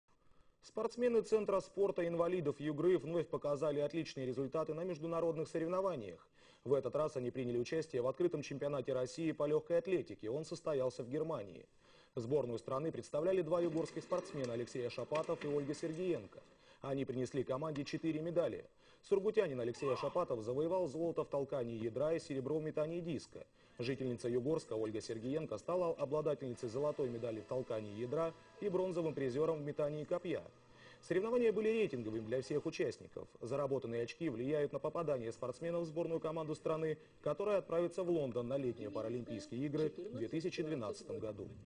Отличное выступление спортсменов-инвалидов
Спортсмены Центра спорта инвалидов Югры вновь показали отличные результаты на международных соревнованиях